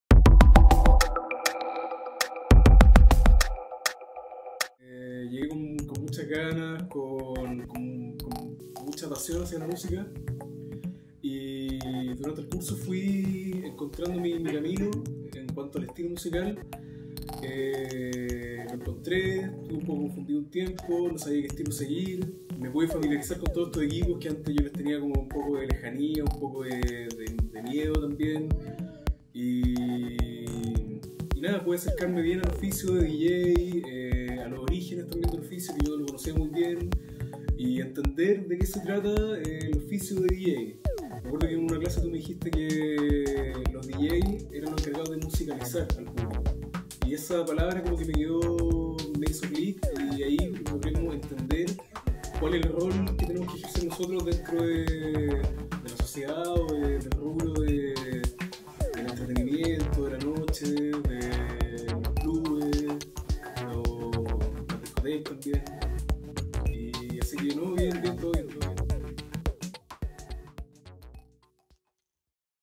Orlando Yupanqui Egresado Dj Profesional
Compartimos el testimonio de nuestro egresado Orlando Yupanqui, egresado del curso Dj Profesional. Aquí nos cuenta su experiencia de lo aprendido en la academia.